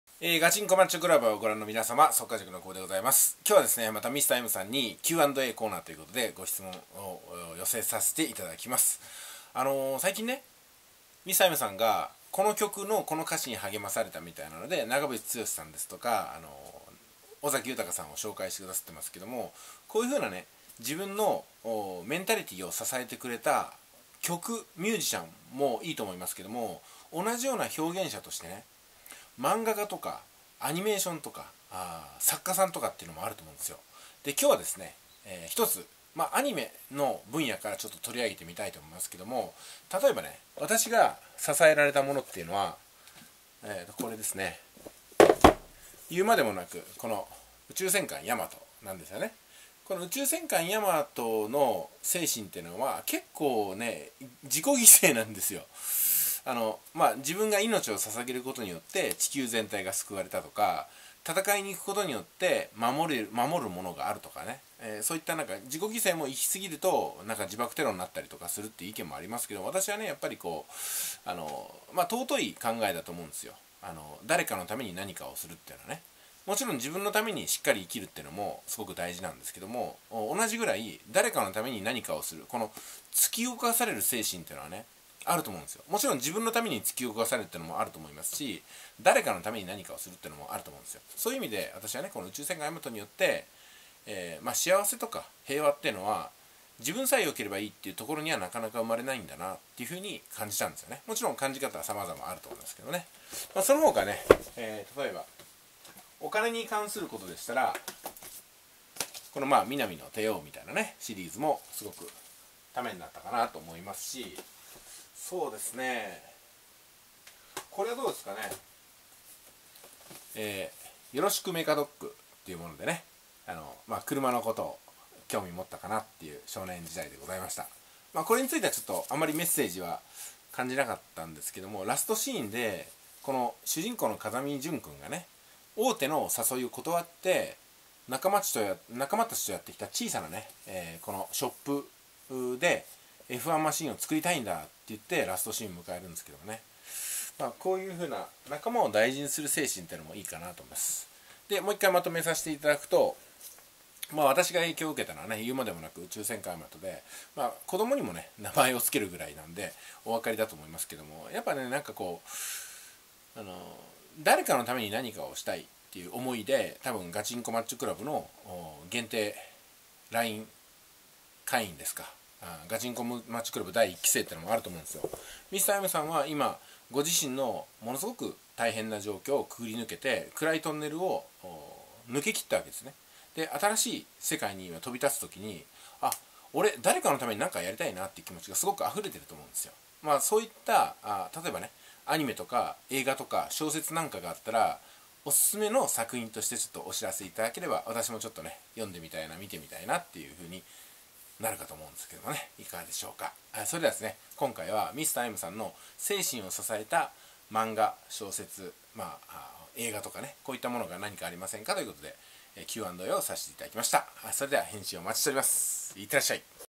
【ガチンコQ&A】宇宙戦艦ヤマト
Ｑ&Ａ 最短でマッチョになる方法とは？元ホームレスが徹底解説！
ミスターノキさんへ【第１６２話 ガチンコマッチョクラブ】